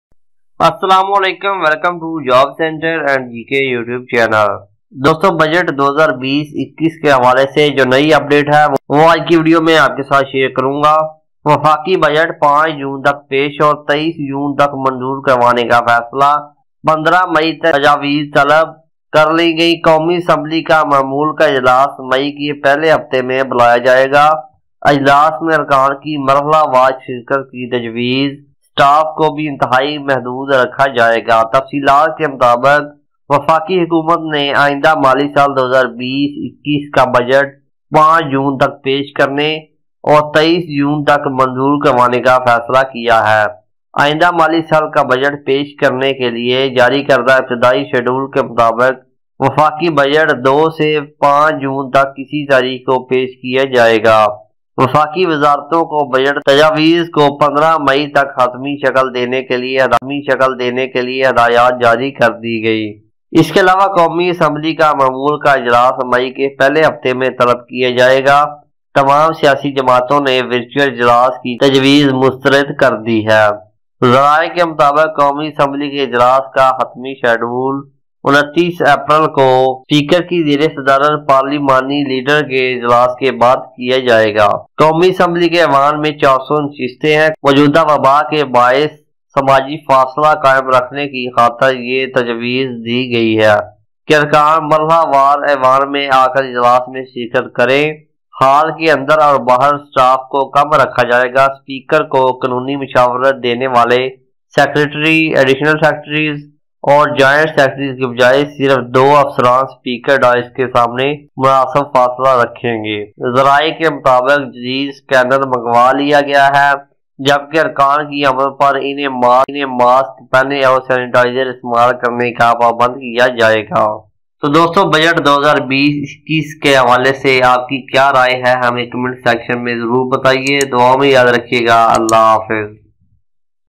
Budget 2020-21 The Latest News Update | Federal Budget 2020 Date
Budget 2020-21 The Latest News Update. In this video I will tell you about all details about budget 2020 and Federal Budget 2020 Date of announcement.